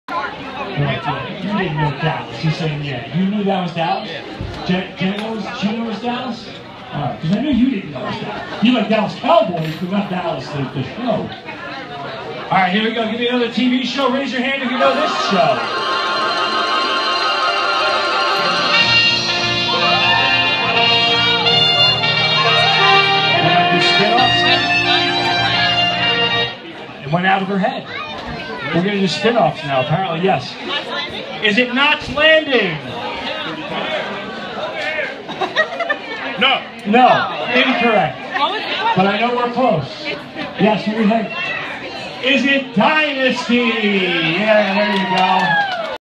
Ray the Prize Guy Playing TV Trivia
Ray has some fun with crowd during a break at the Four Seasons diner in Toms River.